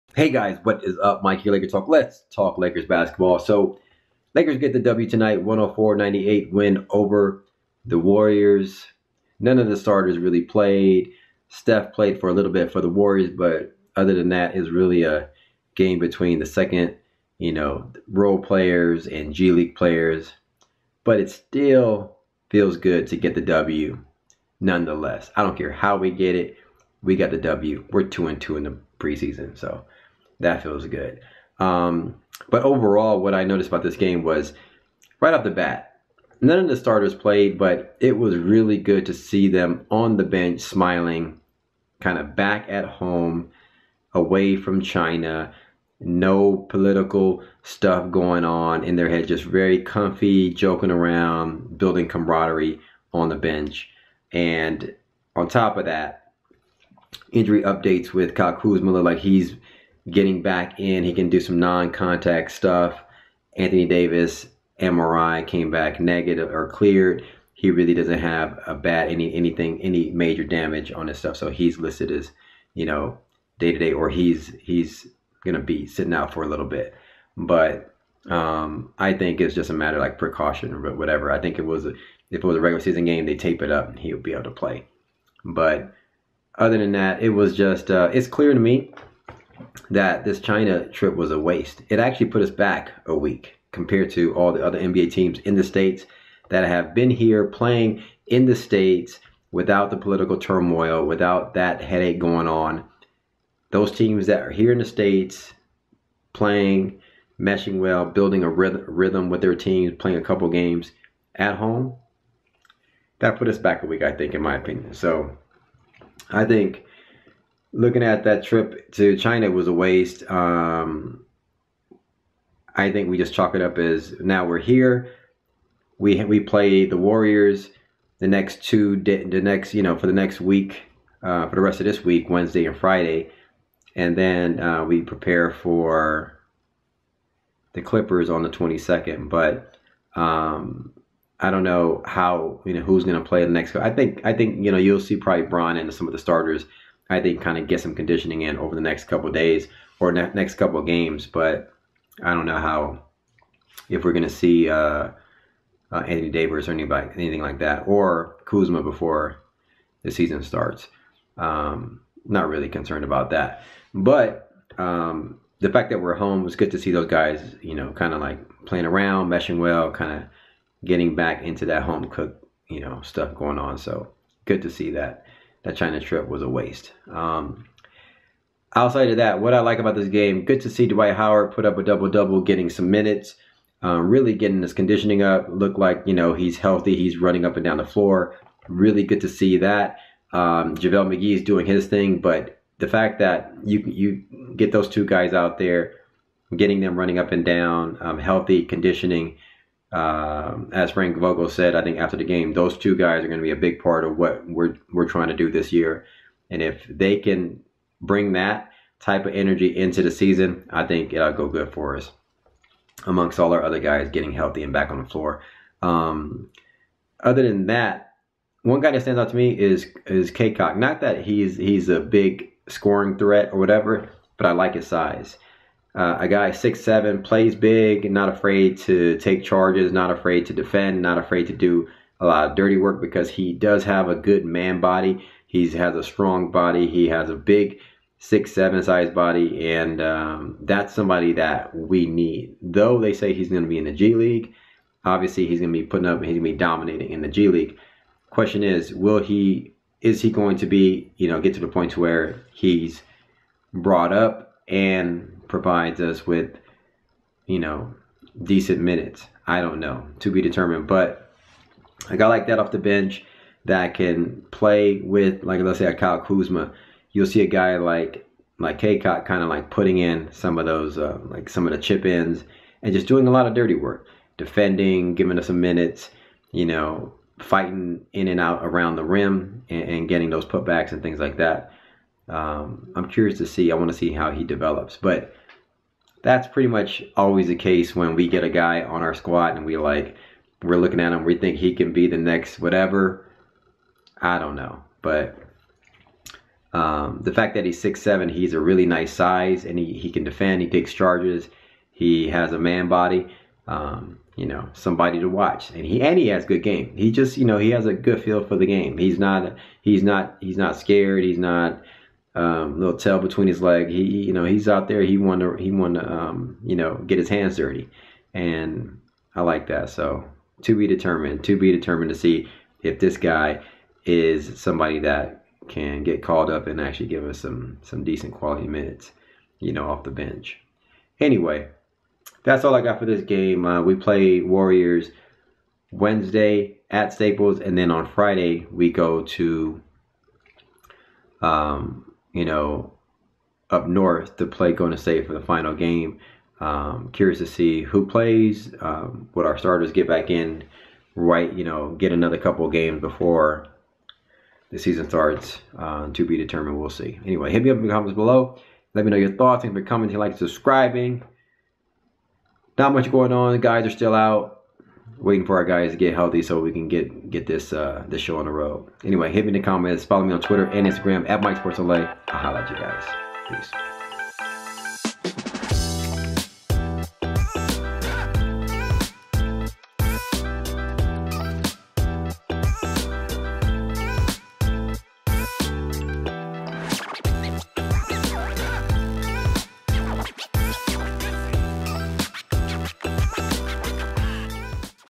Lakers Get the Win, Can Devontae CaCok Develop Into A Role Player for Us?
In this video, I recap Lakers 4th preseason game, Lakers getting the win over the Warriors, Can Devontae Cacok develop into a role player for us?